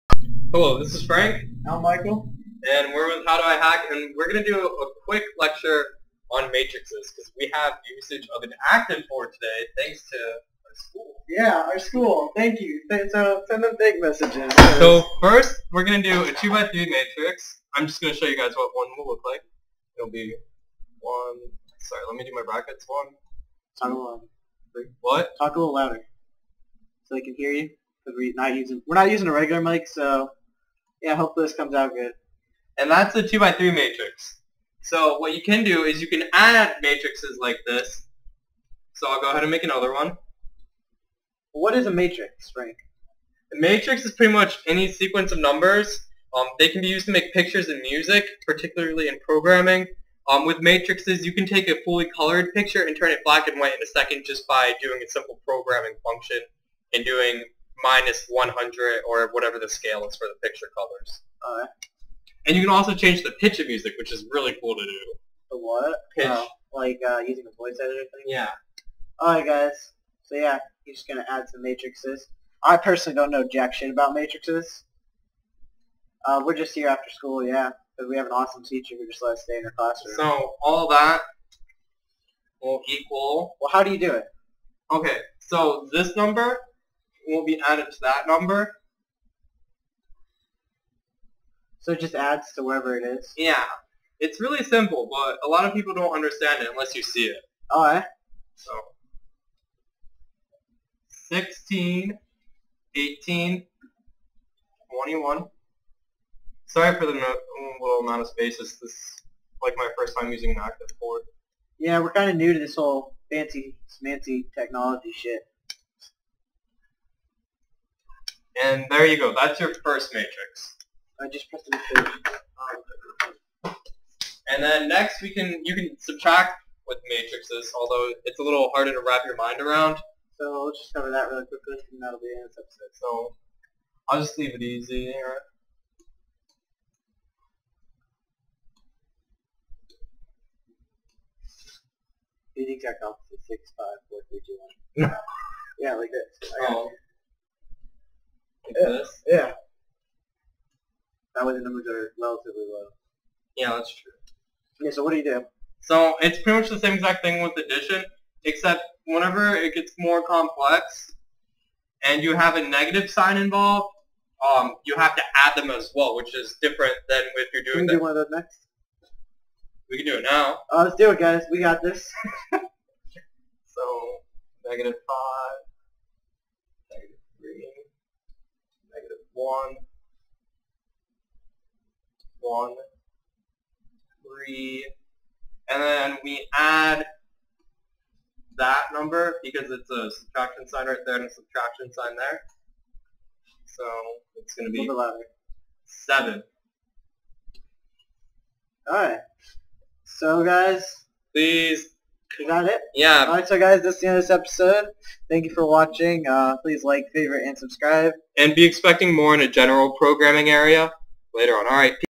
Tutorial 1 - Matrices
Frank and Michael, so mainly Frank, go over addition and subtraction of matrices.  Sorry for the poor quality, we had access to an "ActiveBoard" at our school and wanted to take advantage of this.